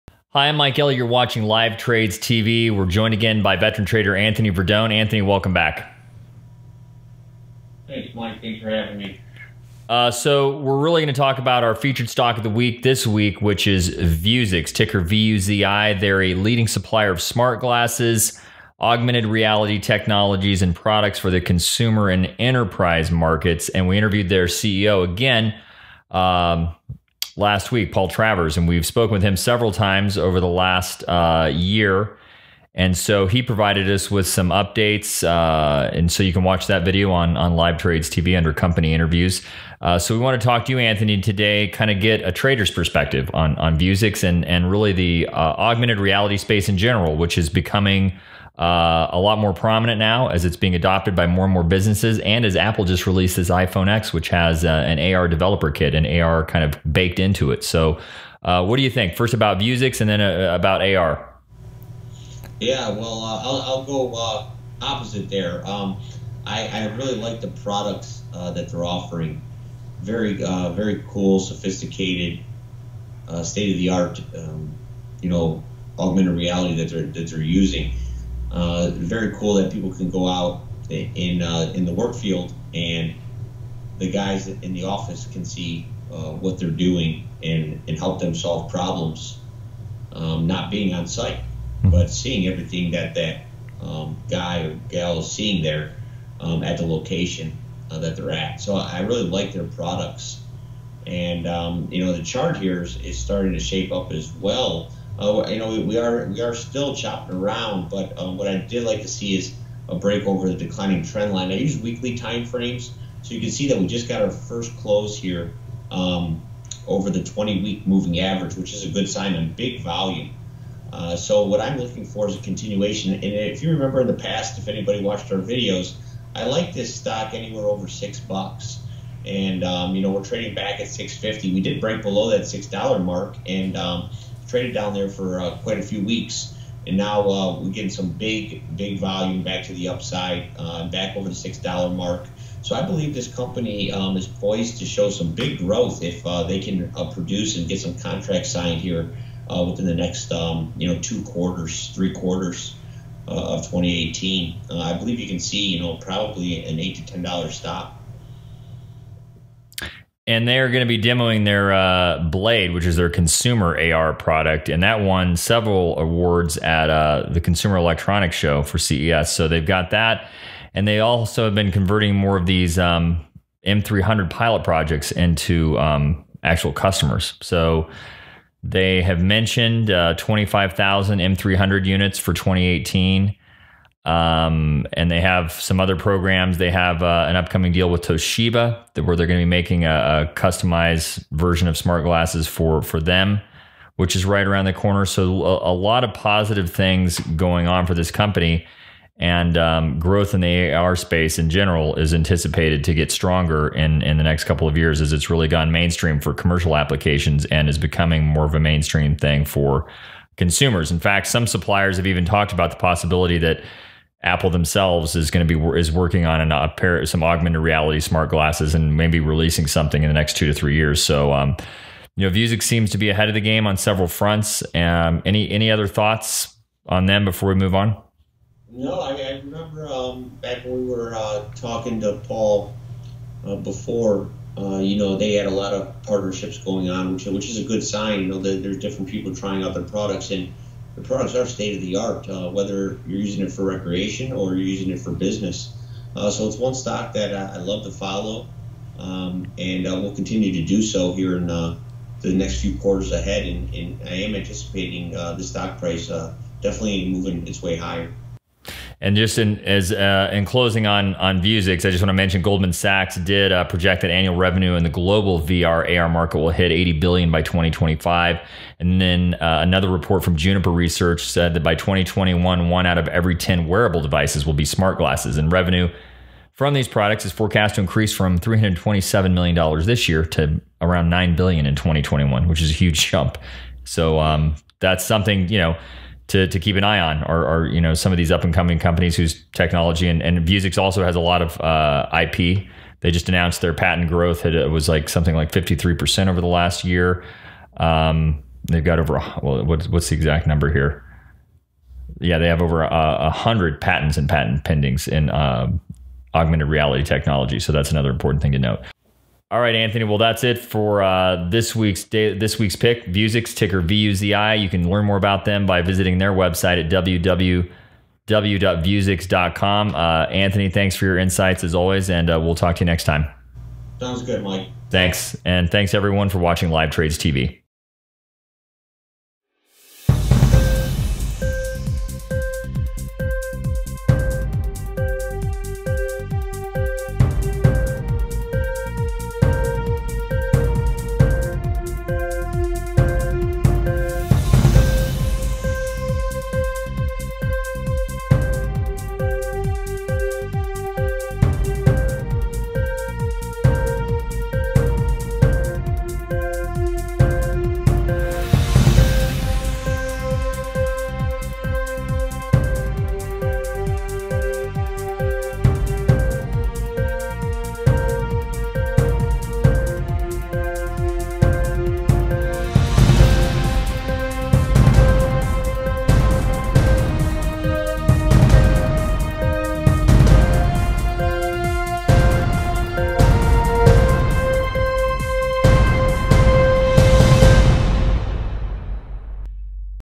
LiveTrades.TV Weekly Picks: Vuzix $VUZI - Leader in AR Smart Glasses
In this episode of the LiveTrades.TV's, we’re joined by veteran trader, Anthony Verdone to discuss Vuzix, a leading supplier of Smart Glasses, Augmented Reality technologies and products for the consumer and enterprise markets which trades under ticker VUZI.